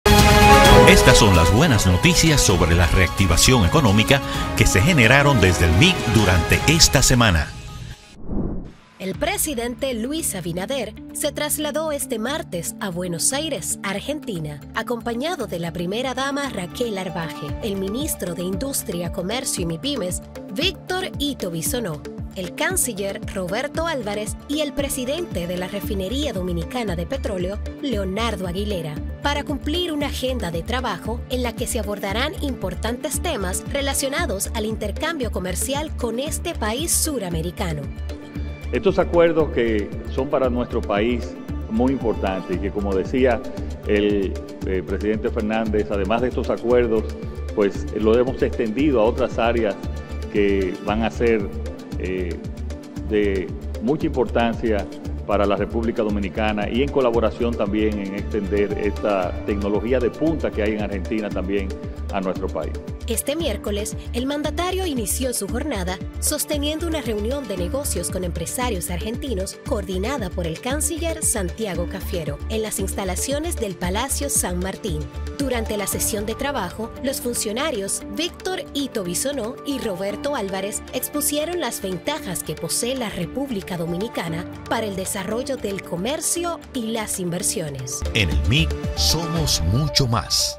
¡Buenas Noticias MICM!
Los presidentes de República Dominicana, Luis Abinader (@LuisAbinader) y de Argentina, Alberto Fernández (@Alferdezok), suscribieron este miércoles un acuerdo de cooperación para el desarrollo hidrocarburífero, que tiene por objeto fomentar y desarrollar la cooperación bilateral entre ambos países.